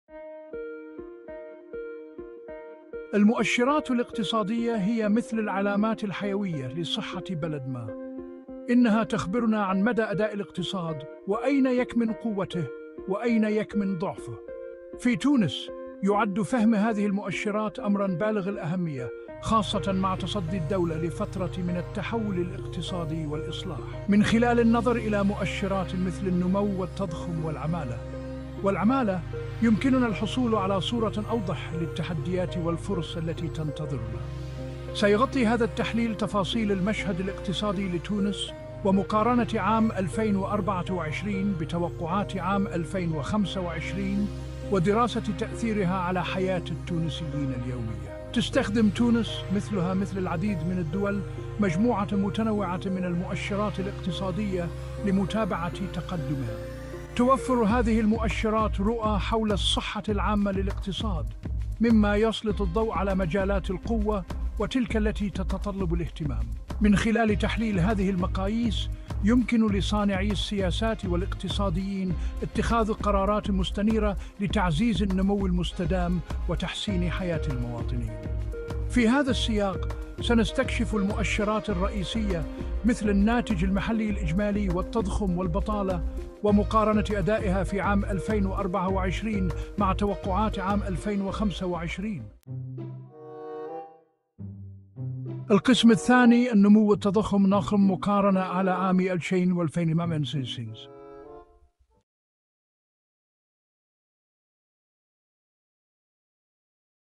قيس سعيد تحت المجهر: المؤشرات الاقتصادية التونسية 2024-2025
في خضم التغيرات الدراماتيكية التي تشهدها تونس، نستعرض في هذا الفيديو مؤشرات الاقتصاد الوطني بين عامي 2024 و2025. مع النمو الذي سجّل 1.2% في 2024 ومعدل تضخم بلغ 9.8%، يبرز السؤال الأهم: هل ستنجح حكومة قيس سعيد في تحسين الوضع الاقتصادي للمواطن التونسي؟

سنحلل الوضع المالي المتأزم، حيث عجزت الميزانية عن الوصول إلى الأرقام المثالية، مما استدعى الإصلاحات الضريبية الجديدة. ولكن، ما هي التبعات الحقيقية لهذه الخطوات على حياة المواطن؟ نُقدّر أن تكاليف المعيشة ستزيد بنسبة تتراوح بين 5% و7%، مما يضع مزيدًا من الضغوط على الأسرة التونسية.

في سوق العمل، سنكشف كيف أن معدل البطالة الذي وصل إلى 16.2% في 2024 قد يستقر عند 15.8%، مع خلق فرص جديدة في قطاعات التكنولوجيا والسياحة. ولكن هل يكفي هذا لتحسين الأوضاع الاجتماعية؟

وبينما يتسارع الدين العام نحو 80% من الناتج المحلي الإجمالي، نتساءل: هل ستنجح الحكومة في تخفيض هذه النسبة إلى 78% في 2025 بفضل الإصلاحات الجديدة؟

وفي ختام هذا التحليل، سنقدم توصيات حيوية للمواطنين لمواجهة هذه التحديات. هل ستتمكن تونس من استعادة عافيتها الاقتصادية في ظل قيادة قيس سعيد؟